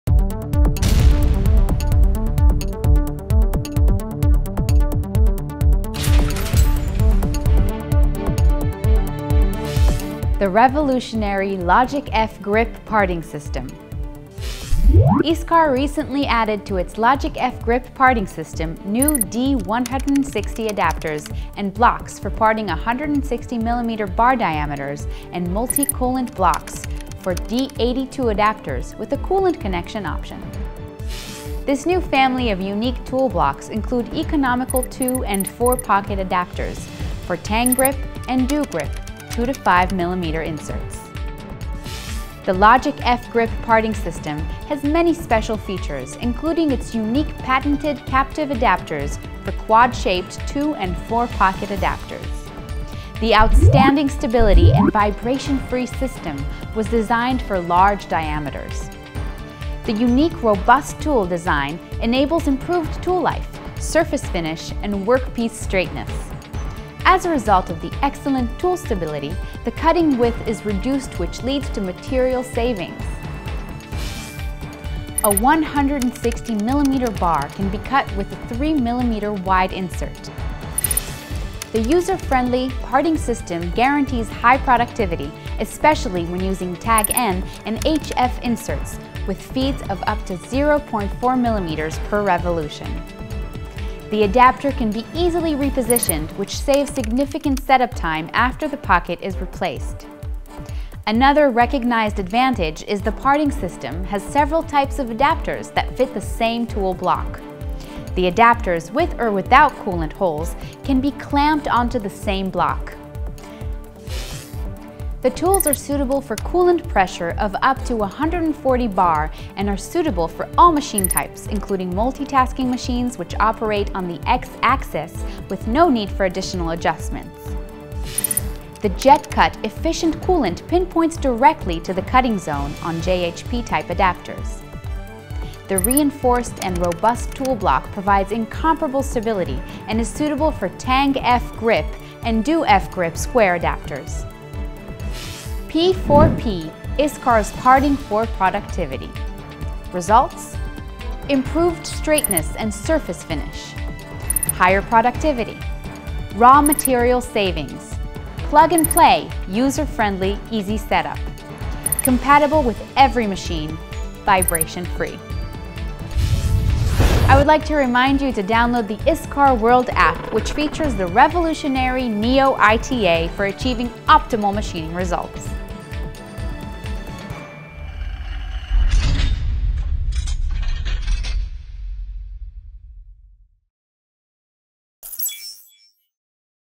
ISCAR TECH TALK - LOGIQFGRIP
ISCAR recently added new intermediate holders to its line of Y-Axis parting tools that feature square TANG-GRIP and DO-GRIP adapters for parting up to 82mm bar diameters.

One of the new parting tools is the LOGIQ-F-GRIP Y AXIS intermediate holder designed for highly productive parting operations on Multi-Tasking machines.  This new parting system is intended for parting at high feed rates in Y-axis and is also an option for parting in an X-Axis direction (only on multi-task machines) using the same standard TANG-GRIP and DO-GRIP square adapters.

There are many special features of the new Y-Axis intermediate holder.

* t’s a newly patented intermediate prismatic holder that fits into ISCAR’s standard tool blocks designed for 32mm blades.

* It was designed to carry the standard TGAQ D82 adapters for TANG-GRIP inserts and DGAQ D82 adapters for DO-GRIP inserts.

* It has outstanding stability and is vibration-free when compared to conventional parting methods due to the adapter’s full underneath support.

* Ensures high surface quality and part straightness.

* Its high blade stiffness and rigidity enable very high feed rates and increased productivity.

* Allows economical parting operations up to 82 mm diameter bars with narrower inserts, which contributes to significant material savings.

* Guarantees high productivity, especially when using TAG N…HF inserts with feed of up to 0.4 mm per revolution.

* May also be used in X-Axis orientation on multi-tasking machines (only!)

* Its suitable for all standard TANG-GRIP and DO-GRIP inserts in widths of 2-5 mm.

* There’s NO setup time after pocket replacement.

* Several adapters can be clamped onto one tool block.

* The tools and adapters are designed for JET-CUT cooling of up to 140 Bars.

 * It’s a door opener for a new application niche.

* They are economical adapters with 2 or 4 pockets.

The second new Y-Axis parting tool is the modular TANG-GRIP adapter suitable for existing shanks and MODULAR-GRIP adaptation for turning centers and multi-tasking machines.

The TANG-GRIP Y-Axis modular adapter was designed for high productivity and stability while parting a Y-axis direction on multi-task and turning center machines.  This new adapter fits all standard adaptation MODULAR-GRIP-XL tool pockets using all types of standard TANG-GRIP inserts.

Special features of these tools include:

* Easy setup and user-friendly system.

* Designed to cut up to 82 mm bar diameters with all standard 3- and 4 mm TANG-GRIP inserts.

* Outstanding stability and vibration-free when compared to conventional parting methods due to the adapter’s full underneath support.

* Guarantees high productivity especially when used with TAG…N HF inserts with feed of up to 0.4mm per revolution.

* Fits all existing modular grip XL adaptations.

* Allows economical parting operations with narrower insert, which contributes to significant material savings.

* The adapters are designed for JET-CUT cooling of up to 140 bars.

* The TAGPAD Y-AXIS adapter setup is identical to any standard parting tool.

* Can be offered for parting up to 125mm diameter as a semi-standard option.

In addition to the newly introduced tools is the new TANG-GRIP (TAG N-HF) type insert designed for high feed parting.
The new durable TAG N-HF inserts are suitable for machining at extremely high feeds. They provide a significant increase in productivity (doubled in most cases) when compared to existing parting inserts available in the market today.

These new inserts feature a reinforced cutting-edge design for extremely high feeds, they can improve productivity by up to 300% and can be used on all standard tools.

I would like to remind you to download the ISCAR World app which features the revolutionary NEO-ITA for achieving optimal machining results.